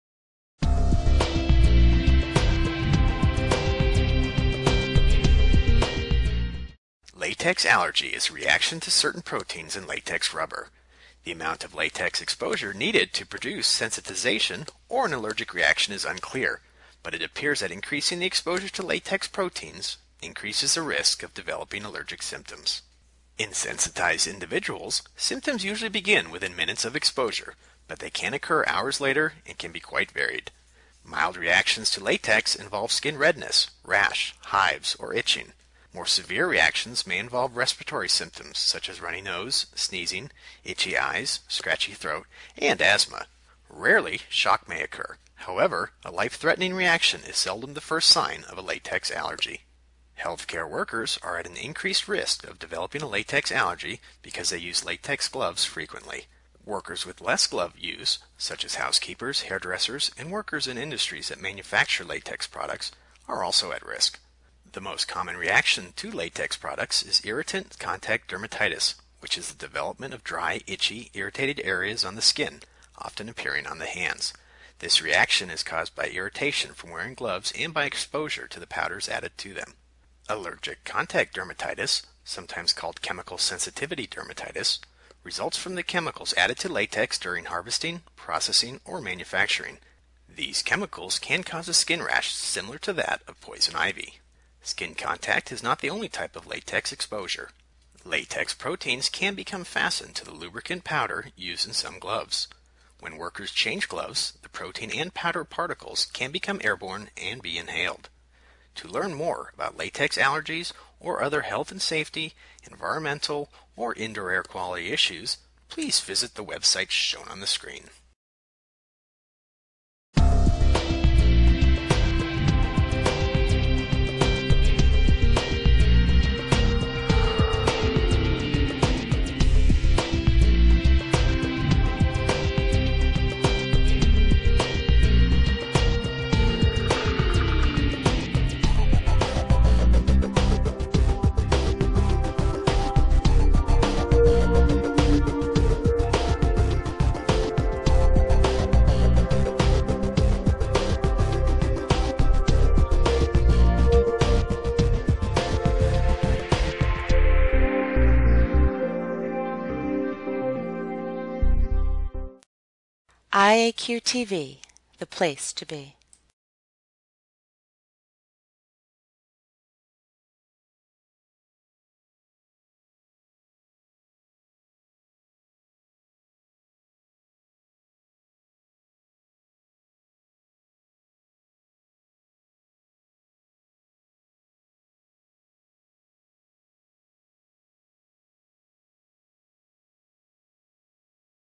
Latex Allergies: What You Need to Know
Latex allergy is a reaction to certain proteins in latex rubber. The amount of latex exposure needed to produce sensitization or an allergic reaction is unclear, but it appears that increasing the exposure to latex proteins increases the risk of developing allergic symptoms.

In sensitized individuals, symptoms usually begin within minutes of exposure; but they can occur hours later and can be quite varied. Mild reactions to latex involve skin redness, rash, hives, or itching. More severe reactions may involve respiratory symptoms such as runny nose, sneezing, itchy eyes, scratchy throat, and asthma. Rarely, shock may occur; however, a life-threatening reaction is seldom the first sign of a latex allergy.